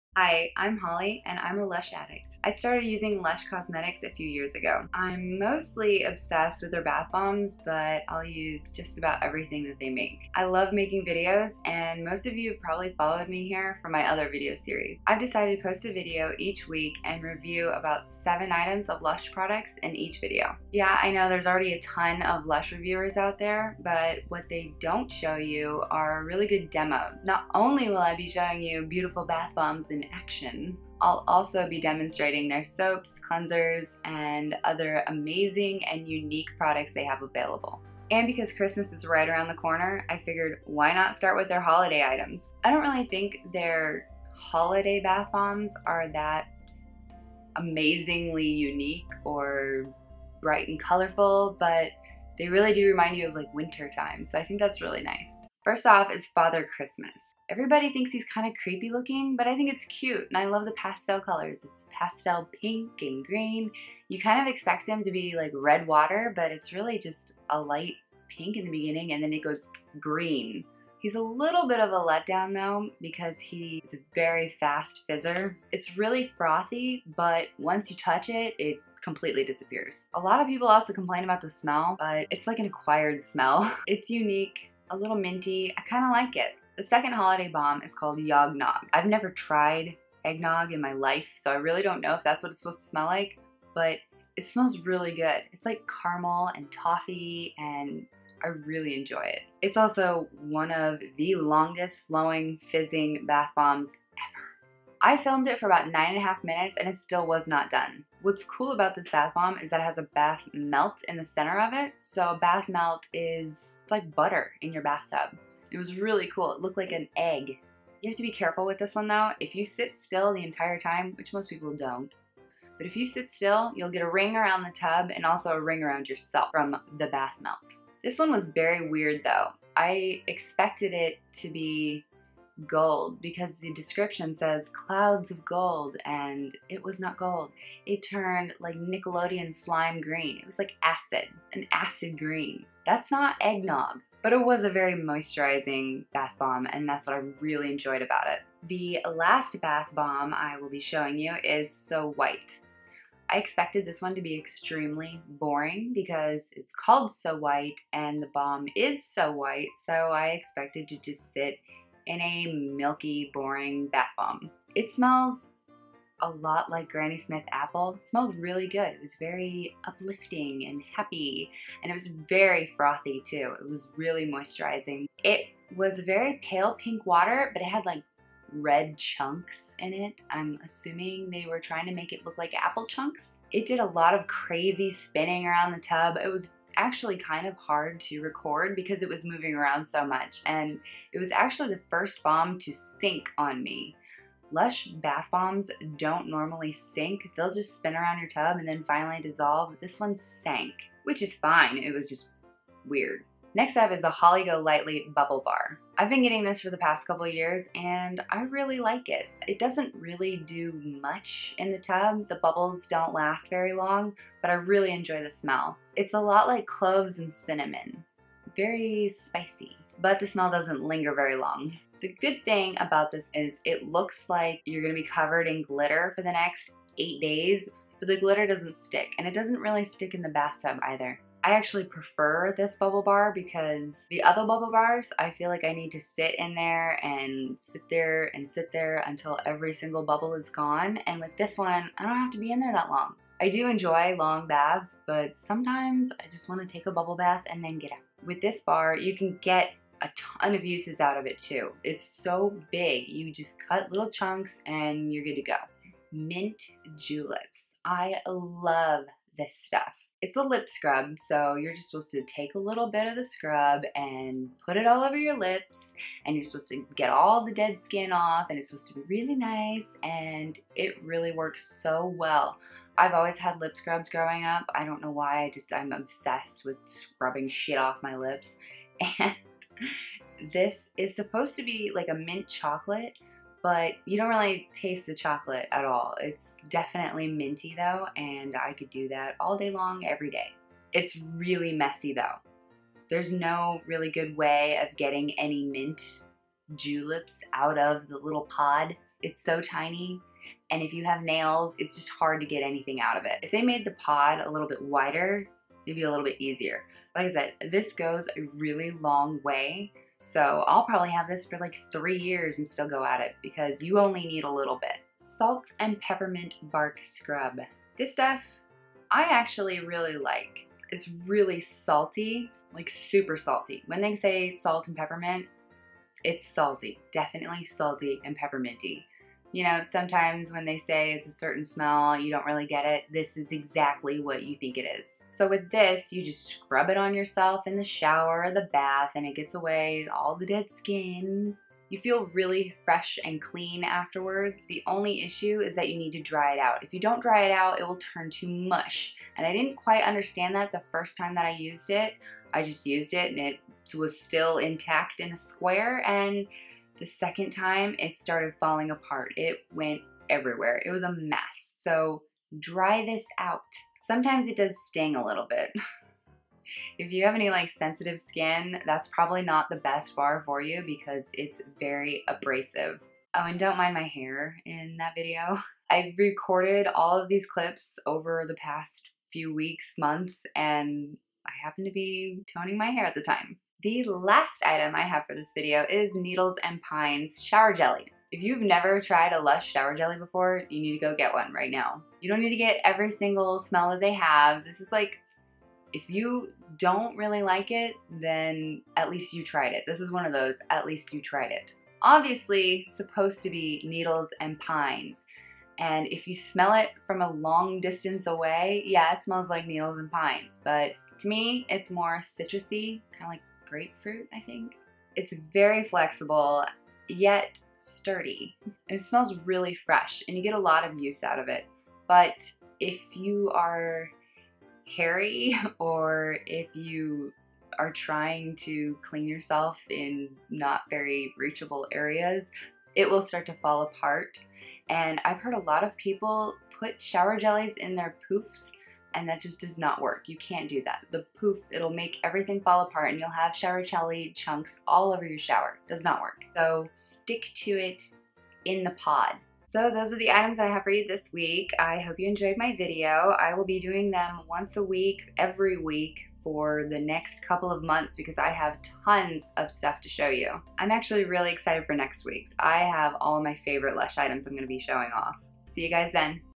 Lush Cosmetics Christmas 2015 Demos & Reviews
I have over 80 LUSH Cosmetics products that I am currently reviewing and making demos for. I'm always buying new items so there will be different & exciting videos each week!

Today I'm talking about a few of LUSH's holiday products:

Father Christmas Bath Bomb
Yog Nog Bath Bomb
So White Bath Bomb
Holly Golightly Bubble Bar
Mint Julips Lip Scrub
Salt & Peppermint Bark Scrub
Needles & Pines Shower Jelly

Footage shot on Nikon D7000